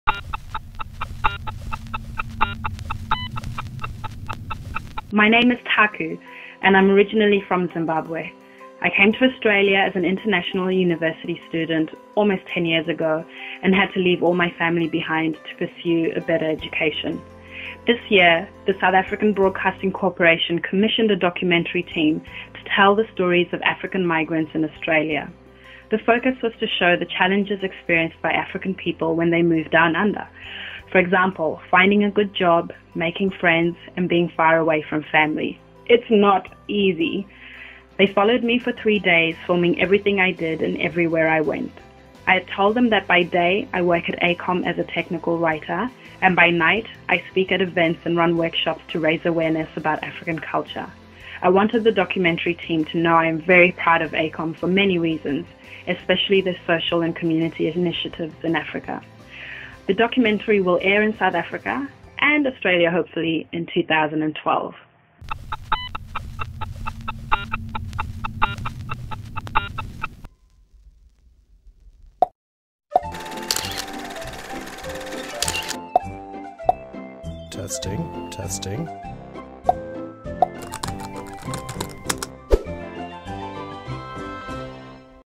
To Australia from Zimbabwe
The South African Broadcasting organization recently commissioned a documentary about African migrants in Australia, and features AECOM's Taku Scrutton.